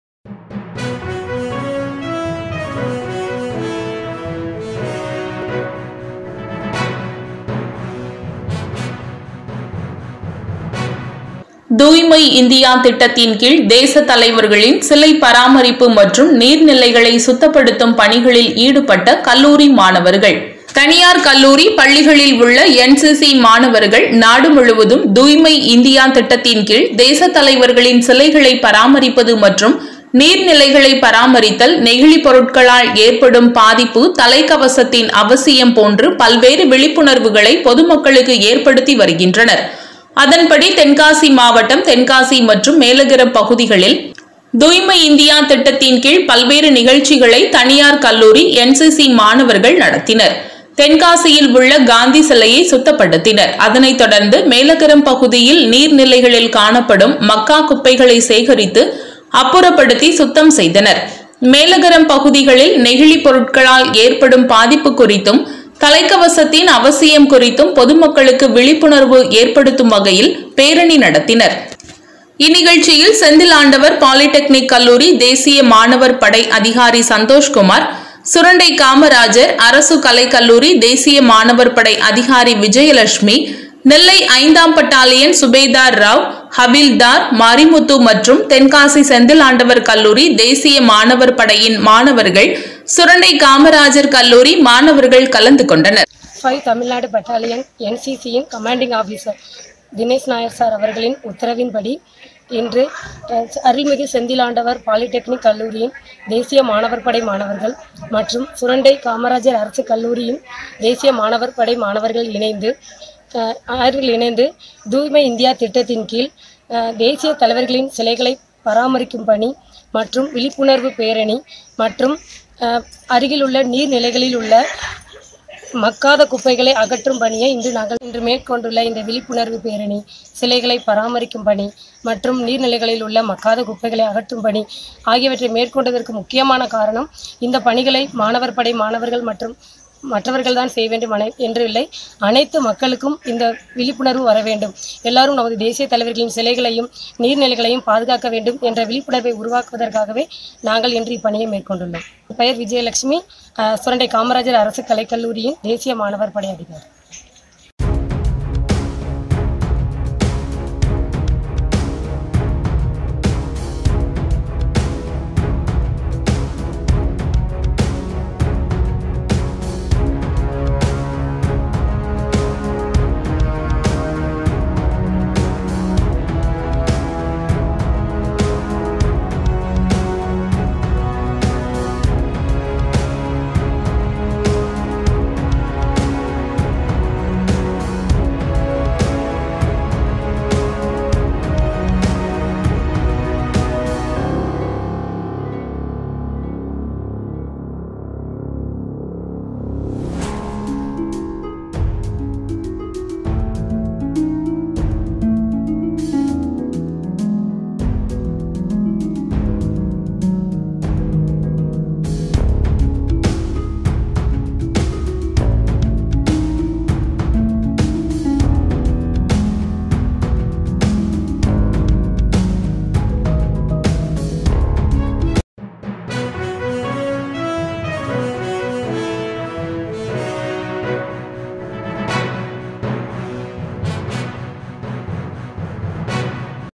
தூய்மைஇந்தியா திட்டத்தின் கீழ் தேசத்தலைவர்களின் சிலை பராமரிப்பு | நீர் நிலைகளை சுத்தப்படுத்தும் பணி
NCC STUDENTS தூய்மைஇந்தியா TENKASI MELAGARAM # திட்டத்தின் கீழ் தேசத் தலைவர்களின் சிலை பராமரிப்பு மற்றும் நீர் நிலைகளை சுத்தப்படுத்தும் பணிகளில் ஈடுபட்ட கல்லூரி மாணவர்கள் .

தனியார் கல்லூரி, பள்ளிகளில் உள்ள  என்சிசி மாணவர்கள் நாடு முழுவதும்  தூய்மை இந்தியா திட்டத்தின் கீழ் தேசத் தலைவர்களின் சிலைகளை பராமரிப்பது மற்றும் நீர் நிலைகளை பராமரித்தல் நெகிழிப் பொருட்களால் ஏற்படும் பாதிப்பு, தலைக்கவசத்தின் அவசியம் போன்று பல்வேறு விழிப்புணர்வுகளை பொதுமக்களுக்கு ஏற்படுத்தி வருகின்றனர். அதன்படி தென்காசி மாவட்டம் தென்காசி மற்றும் மேலகரம் பகுதிகளில் தூய்மை இந்தியா திட்டத்தின் கீழ் பல்வேறு  நிகழ்ச்சிகளை  தனியார் கல்லூரி என்சிசி மாணவர்கள் நடத்தினர். தென்காசியில் உள்ள காந்தி சிலையை சுத்தப்படுத்தினர். அதனை தொடர்ந்து மேலகரம் பகுதியில்  நீர்நிலைகளில் காணப்படும் மக்கா குப்பைகளை சேகரித்து அப்புறப்படுத்தி சுத்தம் செய்தனர், மேலகரம் பகுதிகளில் நெகிழிப் பொருட்களால் ஏற்படும் பாதிப்பு குறித்தும்,  தலைக்கவசத்தின் அவசியம் குறித்தும் பொதுமக்களுக்கு விழிப்புணர்வு ஏற்படுத்தும் வகையில் பேரணி நடத்தினர். இந்நிகழ்ச்சியில் செந்தில் ஆண்டவர் பாலிடெக்னிக் கல்லூரியில் தேசிய மாணவர் படை அதிகாரி சந்தோஷ்குமார், சுரண்டை காமராஜர் அரசு கலைக் கல்லூரி தேசிய மாணவர் படை அதிகாரி விஜயலட்சுமி, நெல்லை ஐந்தாம் பட்டாலியன் சுபேதார் ராவ், ஹவில்தார்  கே.மாரிமுத்து மற்றும் தென்காசி செந்தில் ஆண்டவர் கல்லூரி தேசிய மாணவர் படையின் மாணவர்கள் , சுரண்டை காமராஜர் கல்லூரி மாணவர்கள் கலந்து கொண்டனர்